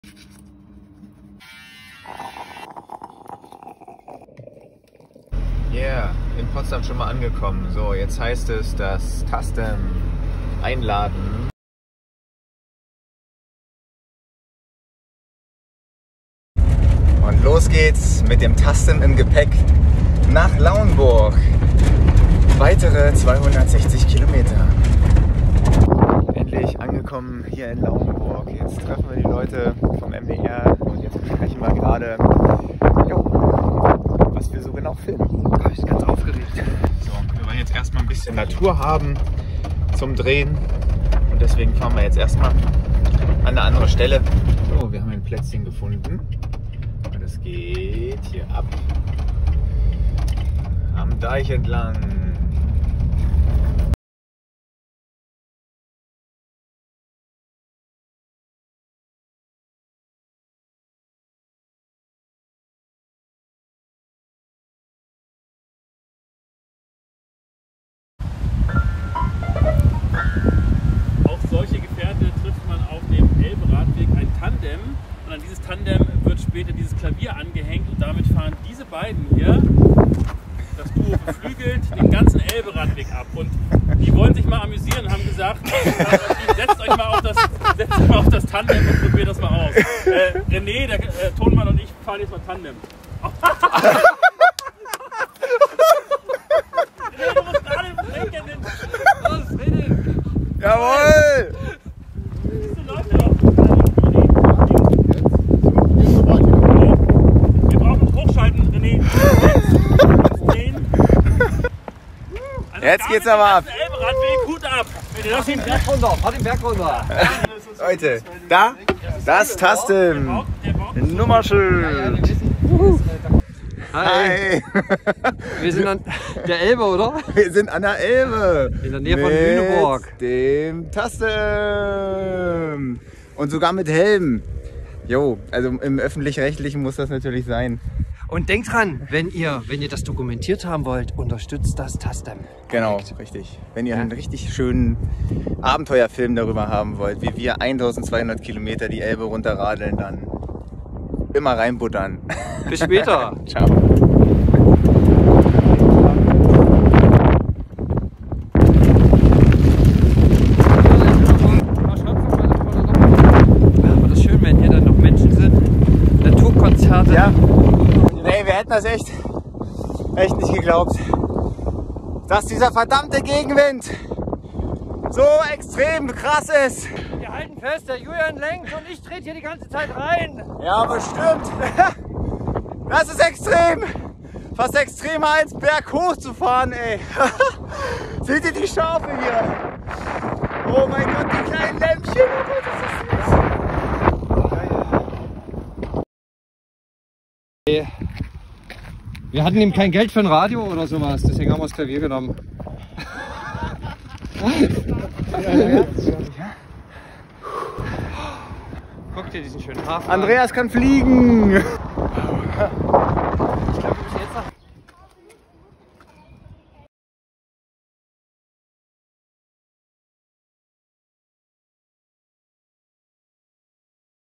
Mit dem "TASTDEM" beim NDR mit Thilo Tautz
Am Freitag haben wir für den NDR-Hanseblick mit Thilo Tautz und Heinz Galling in Lauenburg gedreht. Im Video könnt ihr Thilo sehen, wie er mit dem Tontechniker das erste mal auf einem Tandem sitzt. Sehr witzig! 😃
Am 30.05. könnt ihr uns in der Sendung mit dem Tastdem sehen, erstmals dann wirklich an der Elbe.
Der Gegenwind war enorm, aber die Landschaft traumhaft schön. Jetzt freuen wir uns noch mehr auf den Sommer.
davon, für euch alle Konzerte der Elbetour KOSTENLOS streamen zu können. Danke, dass ihr es möglich macht 🙂